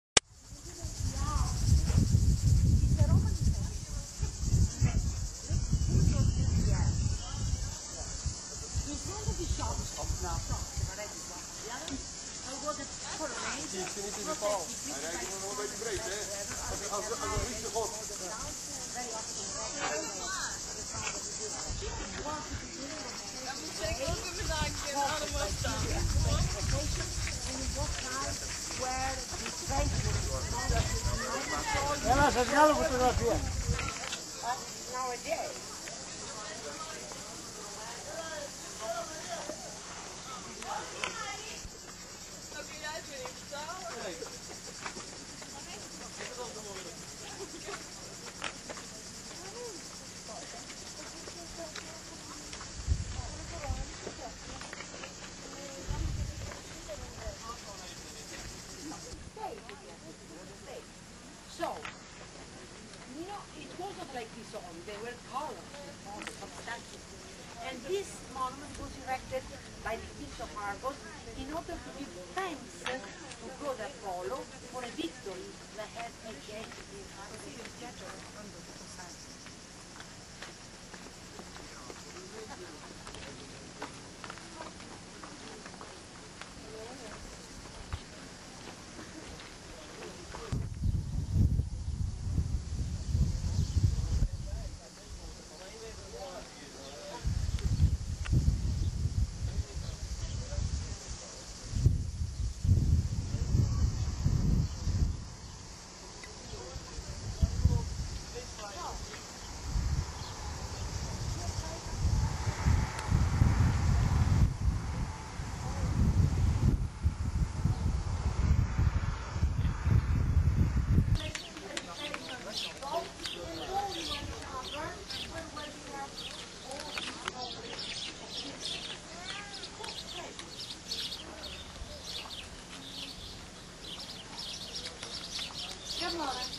Delphi Greece
Delphi in ancient times was considered the center of the known world, the place where heaven and earth met. This was the place on earth where man was closest to God. In Mythology, Delphi was the meeting place of two eagles, released by Zeus and sent in opposite directions. Where they met indicated the center of the earth. Delphi is known as the center of worship for the God Apollo, son of Zeus who embodied moral discipline and spiritual clarity